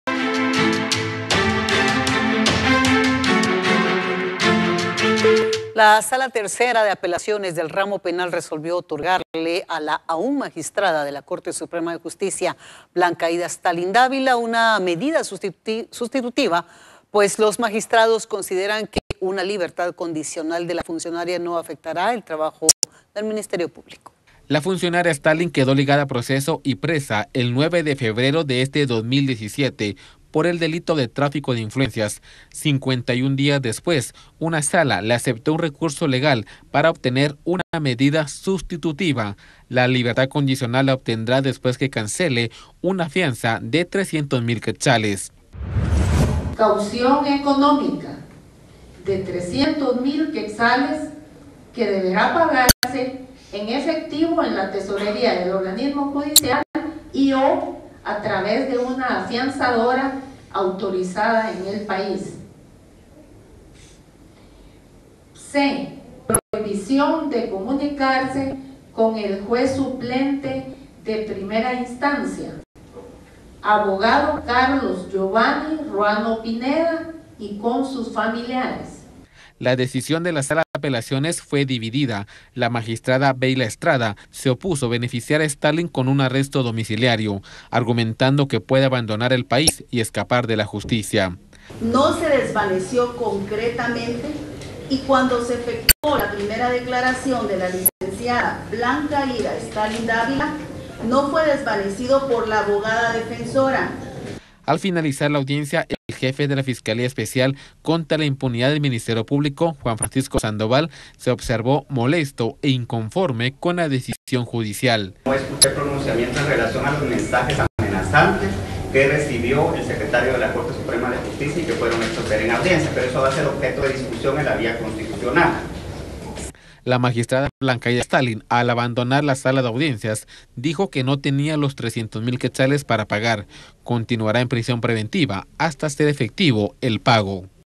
Stalling queda con arresto domiciliario
Tercera Sala de Apelaciones le otorgó medidas sustitutivas